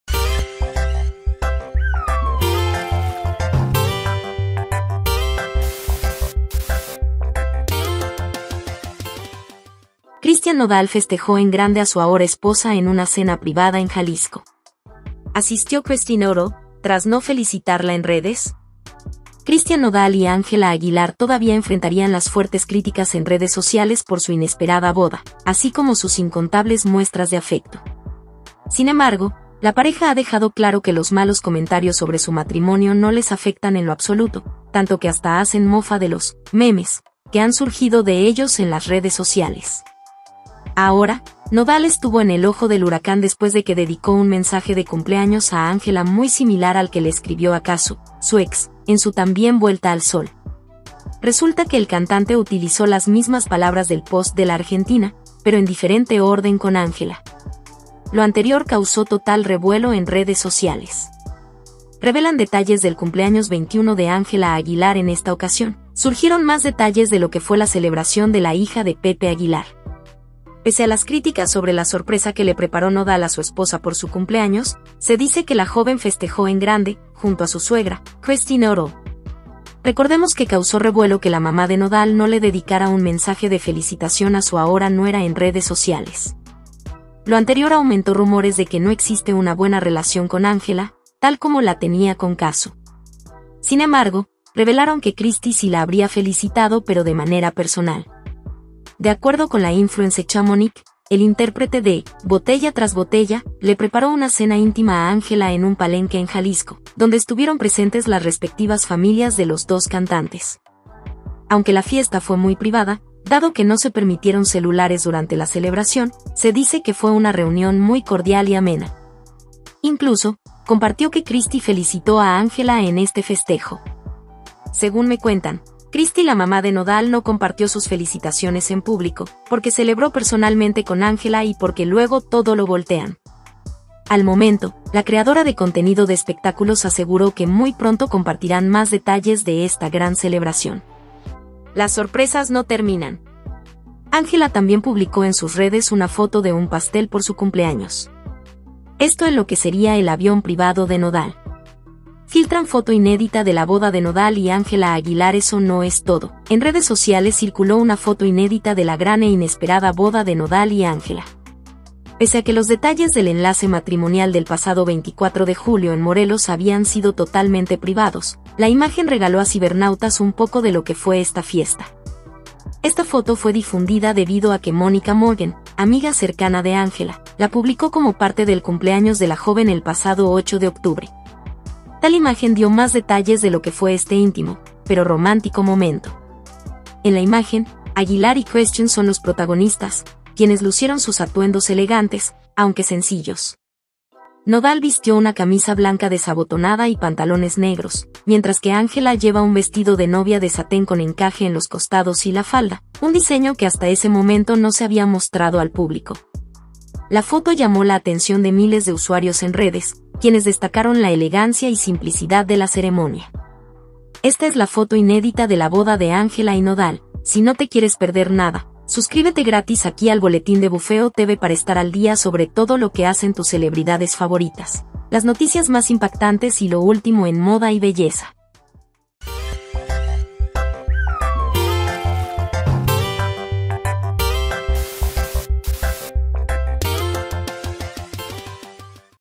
Ángela Aguilar y Nodal Revelan foto inédita de su boda y detalles de la fiesta de cumpleaños de la c
Christian Nodal festejó en grande a su ahora esposa en una cena privada en Jalisco. ¿Asistió Cristy Nodal, tras no felicitarla en redes?
Sin embargo, la pareja ha dejado claro que los malos comentarios sobre su matrimonio no les afectan en lo absoluto, tanto que hasta hacen mofa de los ‘memes’ que han surgido de ellos en las redes sociales.